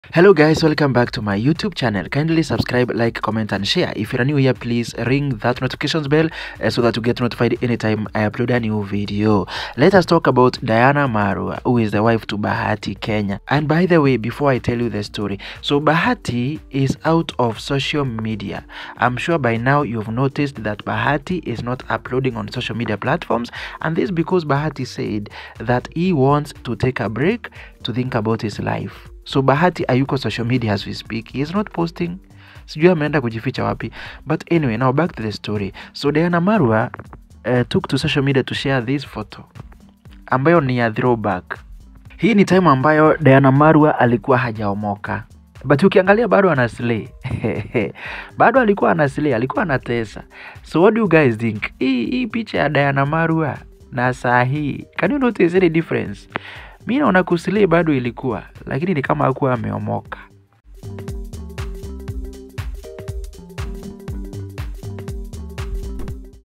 DIANA MARUA SHARES HER THROW BACK PHOTOS. WAH! 😳 PESA NI SABUNI
Diana Marua has shared her throwback photo that has caused mixed reactions on social media.